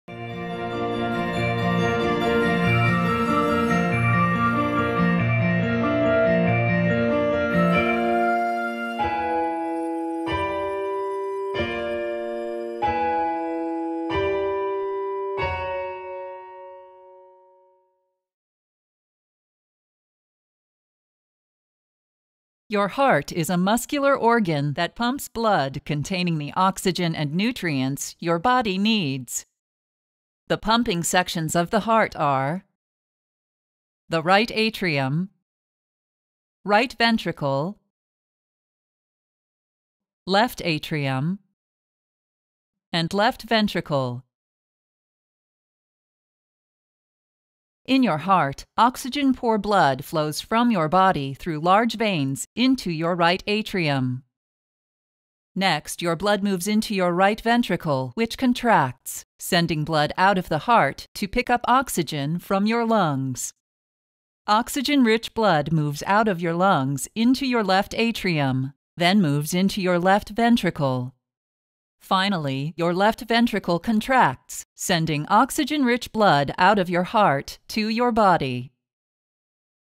Anatomy and Function of the Heart
This 3D medical animation shows normal anatomy of the human heart, and explains the pathway of blood through it.

ANH14129a